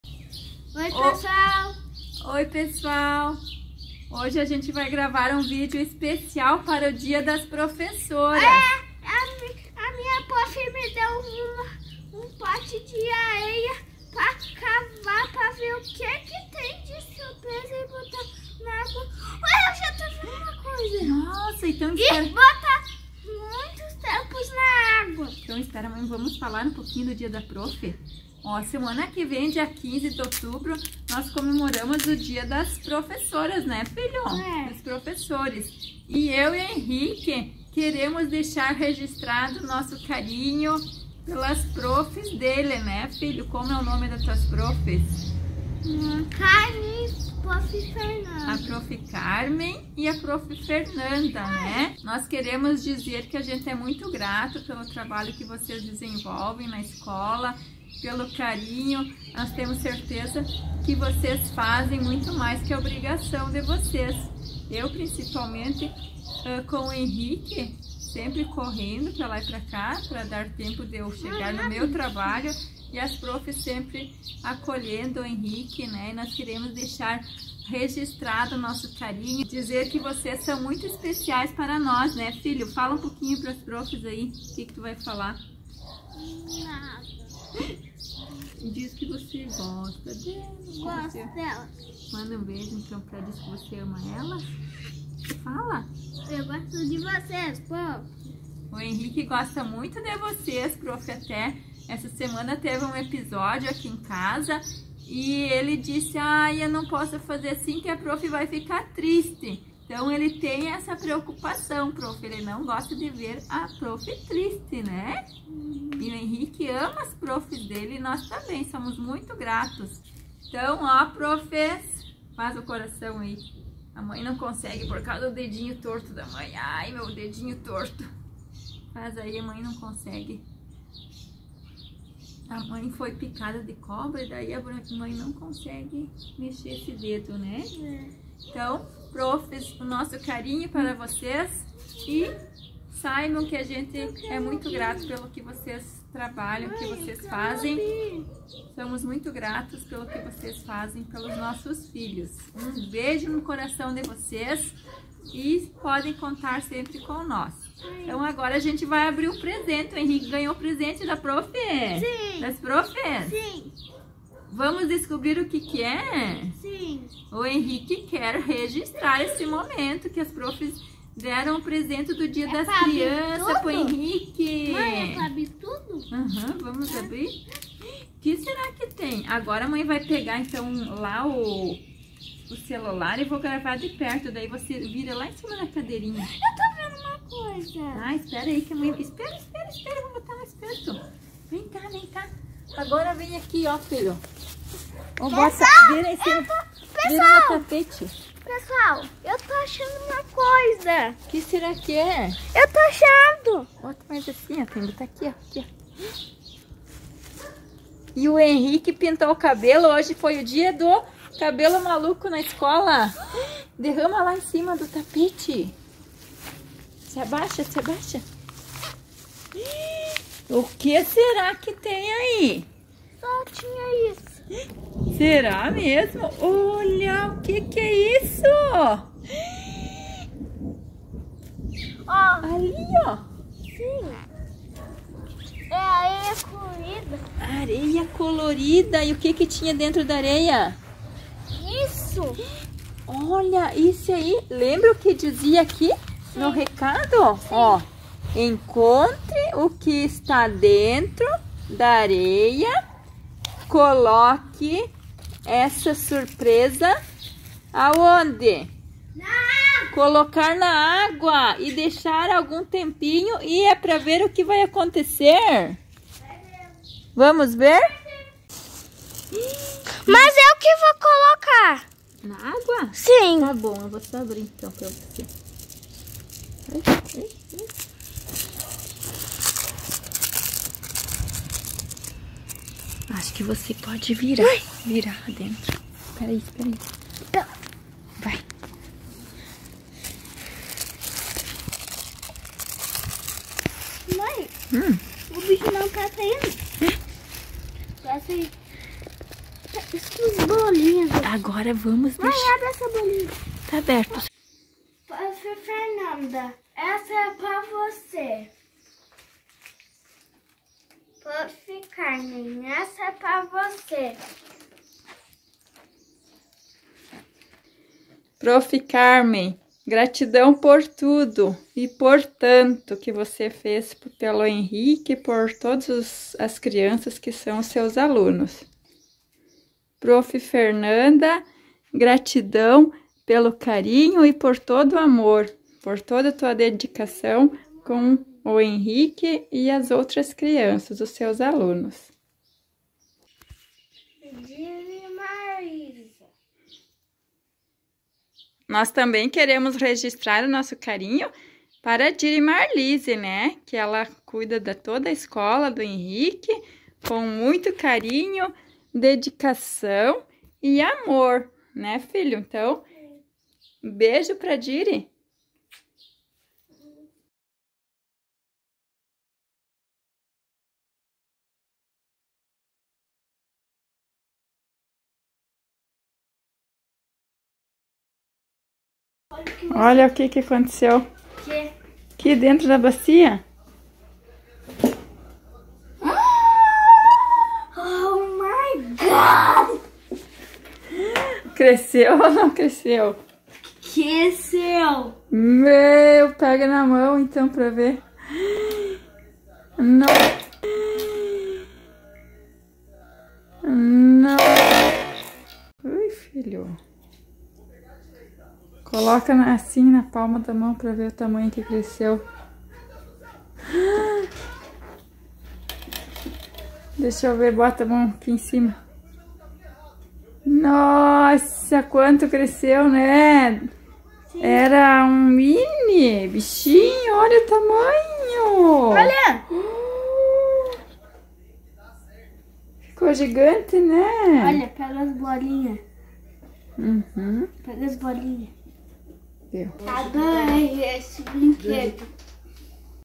PARABÉNS PROFESSOR! Em especial às professoras do Henrique
Professoras Carmem e Fernanda!
Gratidão por tudo! Gratidão Por tanto!
Gratidão pelo seu carinho! Gratidão pelo seu amor!
Abraços do Henrique e família!